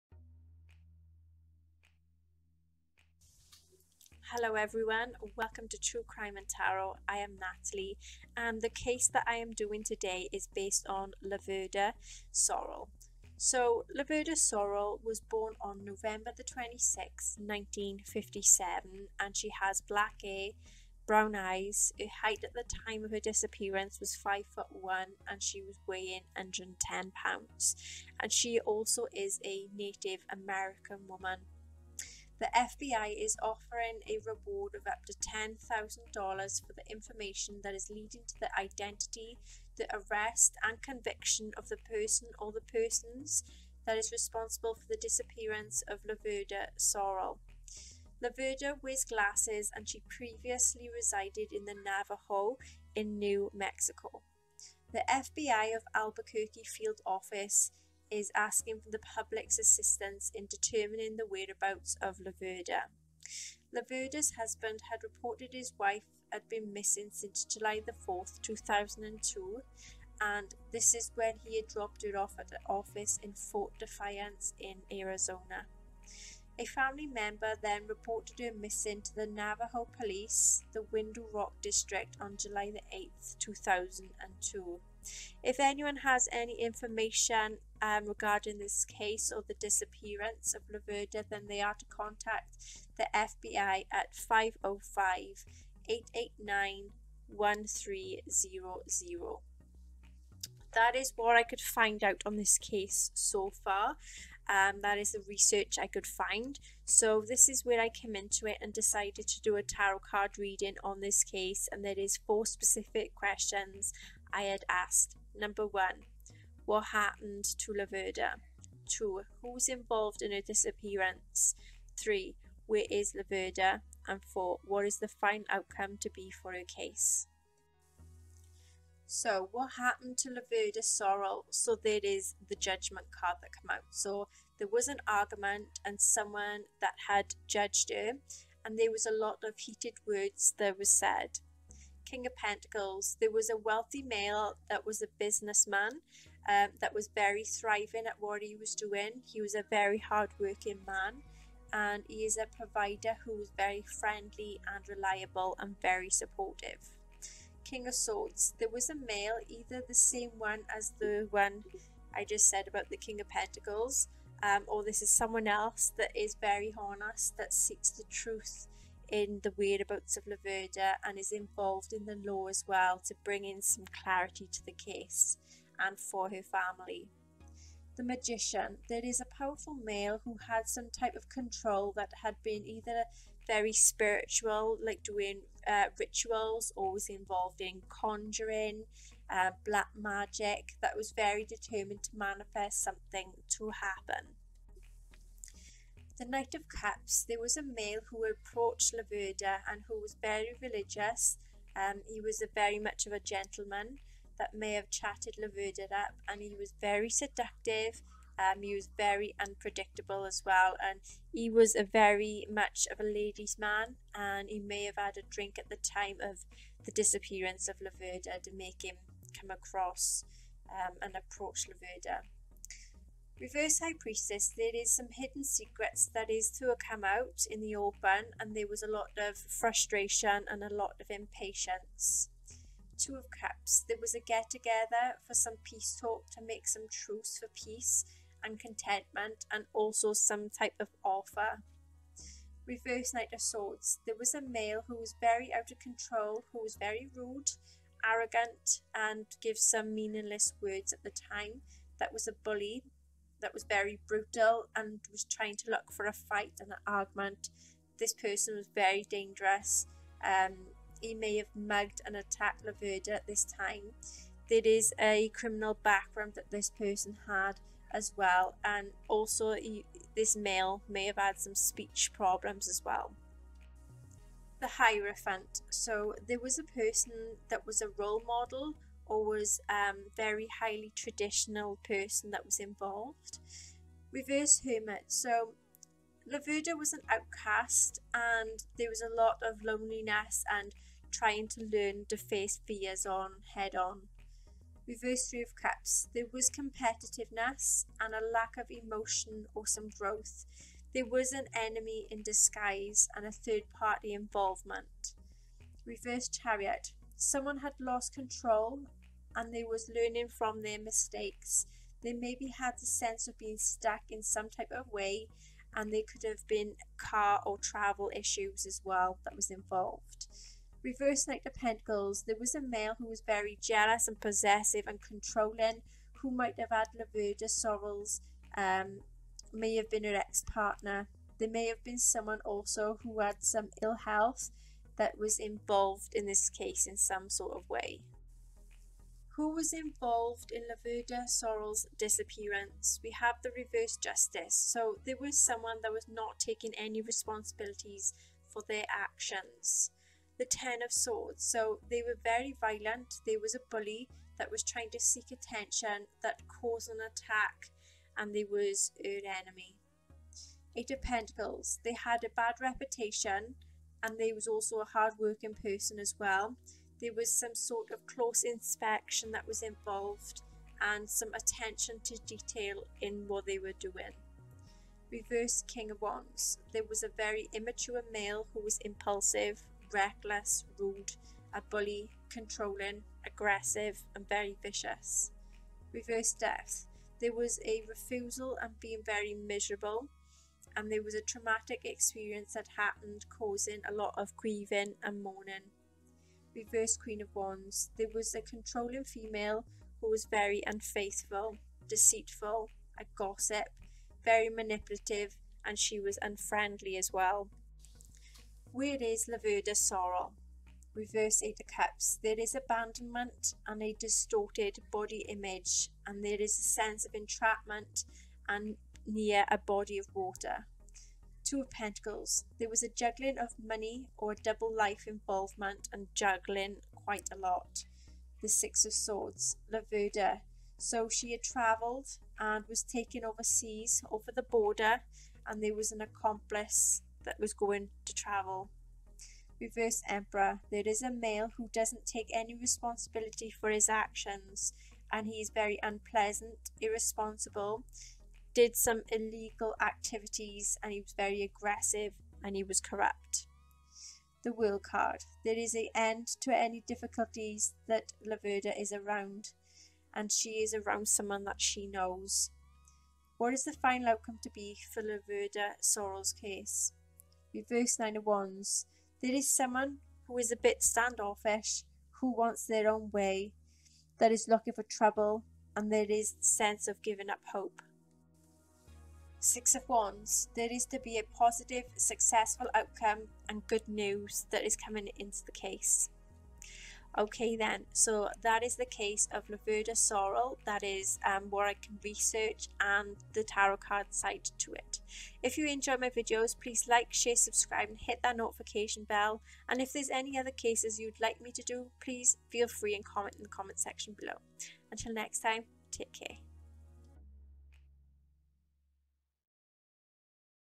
The Case Of Laverda Sorrell
Hello and Welcome to True Crime And Tarot
This is The Case of Laverda Sorrell  . I Researched This Case And I Felt Inclined To Do This Case And Did A Tarot Reading .